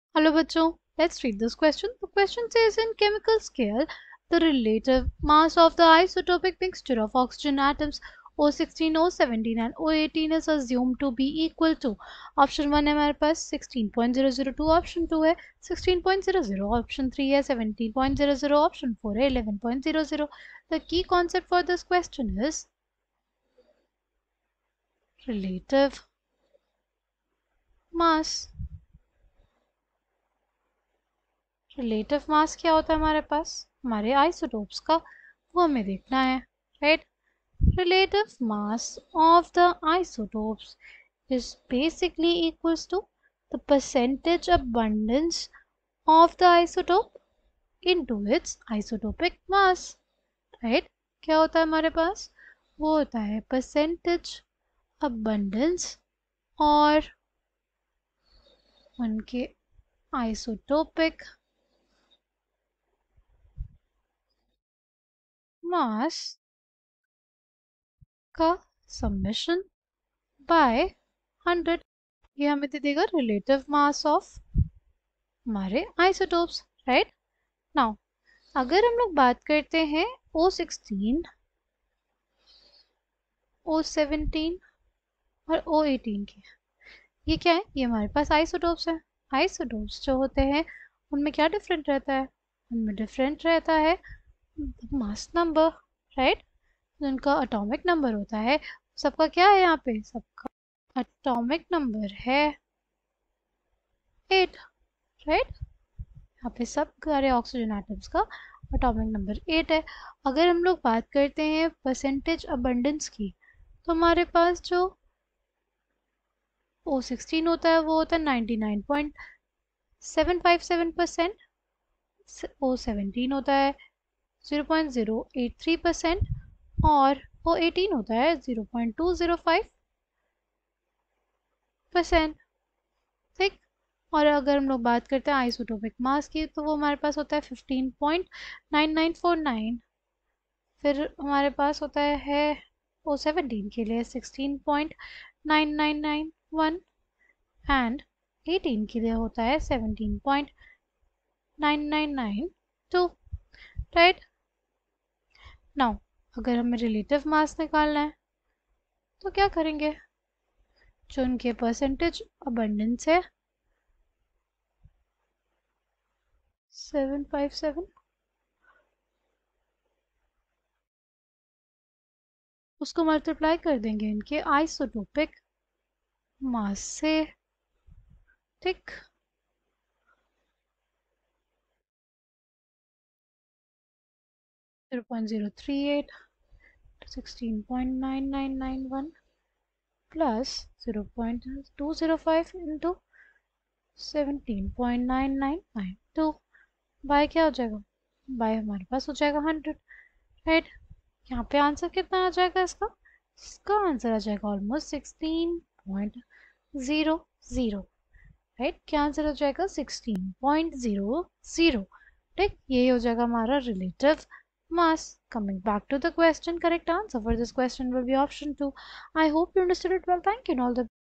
In chemical scale, the relative mass of the isotopic mixture of oxygen atoms \( \left(O^{16}, O^...
In chemical scale, the relative mass of the isotopic mixture of oxygen atoms \( \left(O^{16}, O^{17}, O^{18}\right) \) is assumed to be equal to
(1) \( 16.002 \)
(2) \( 16.00 \)
(3) \( 17.00 \)
(4) \( 11.00 \)